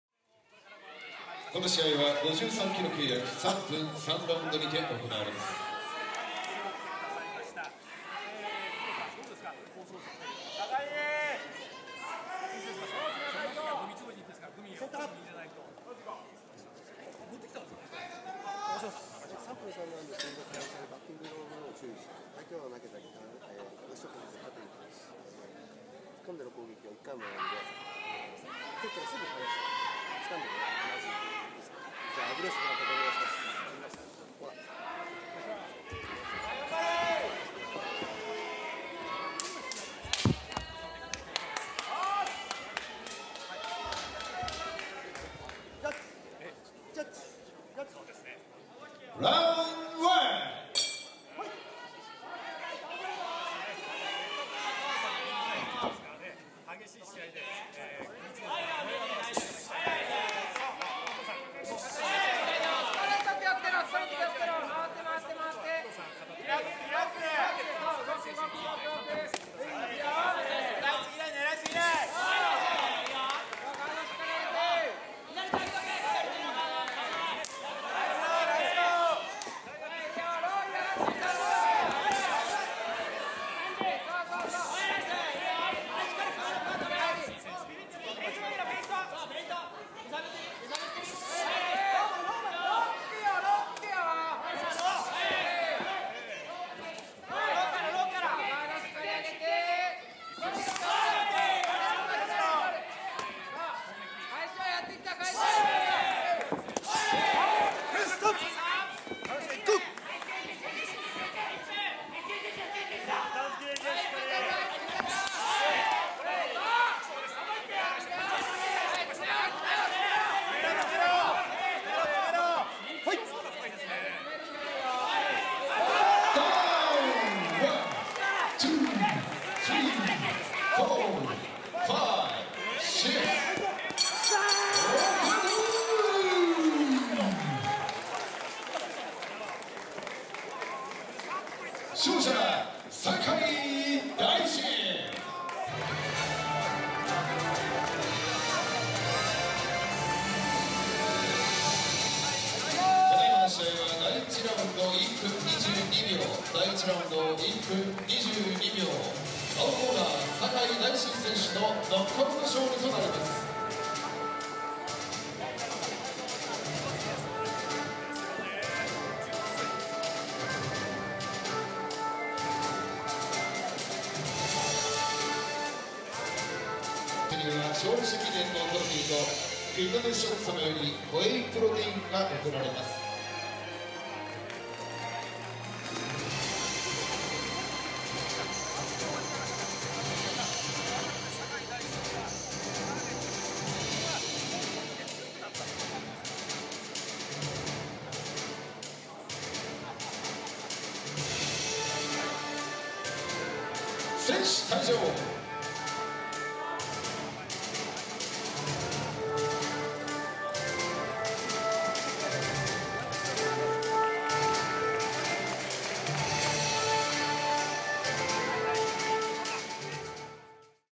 中井将史 vs 栄井大進／Masafumi Nakai vs Daishin Sakai｜2017.10.9【OFFICIAL】
【2017年10月9日 RISE120】
▼-53kg契約　3分3R
中井将史（リアルディール）
栄井大進（TARGET）